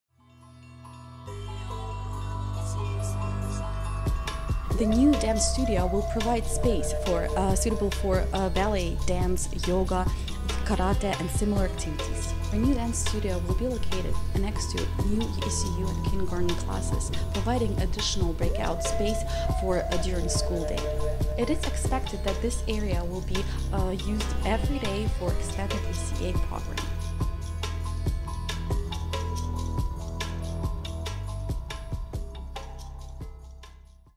Phase II Building Project - Dance Studio with Ms Valeria / 2017
Dance Studio providing a new place for specific sports and activities, as well as extra break-out space for during the school day.

FOLLOW PSI ON SOCIAL MEDIA!